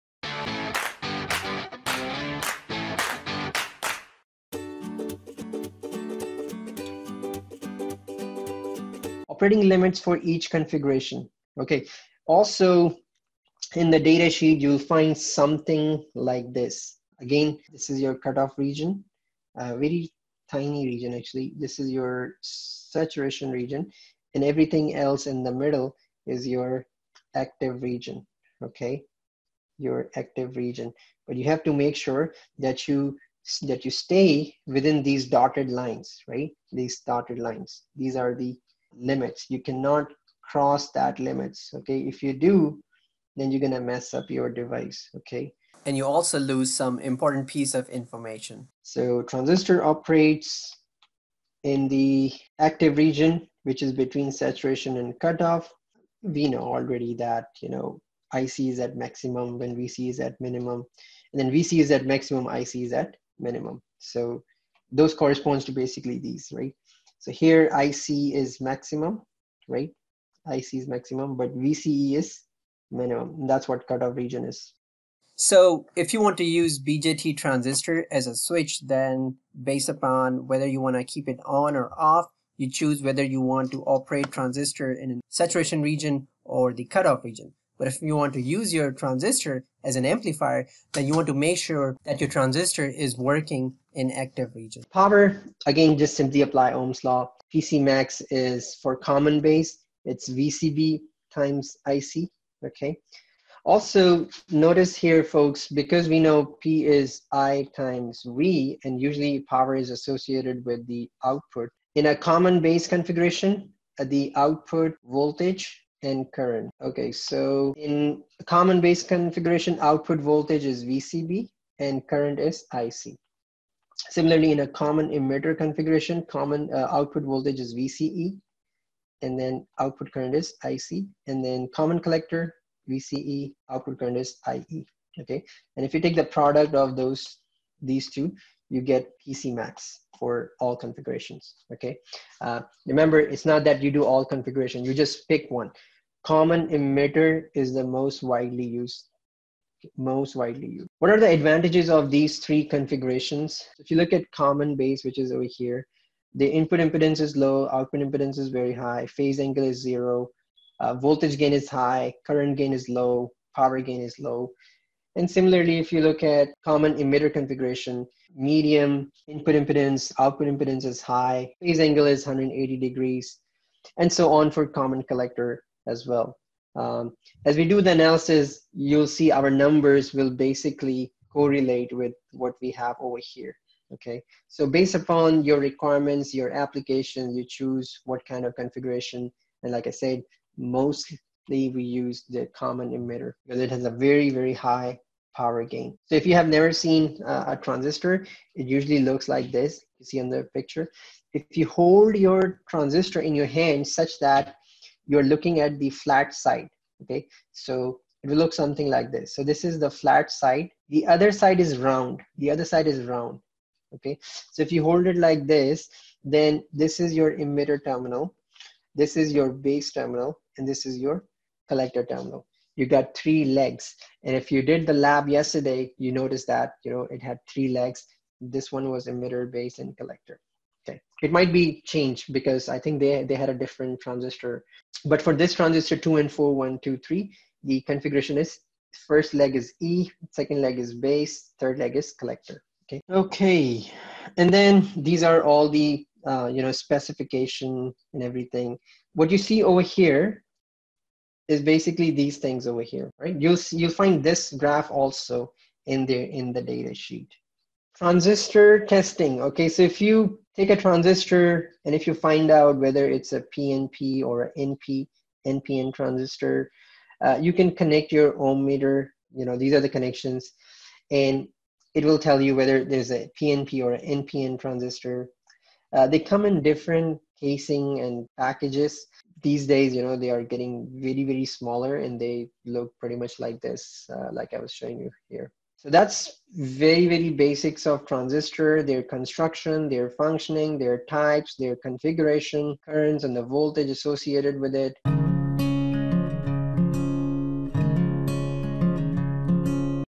#46 Operating Limits of BJT Transistors | Characteristics of CB, CC, CE Configurations
Learn about basics of BJT transistor operations and limits. Learn about power limits, and characteristics for all kind of configurations including CB, CC, and CE.
I suggest you watch previous videos to gain better understanding.
Credit: 👍
        -  Sound 🎵 : Camtesia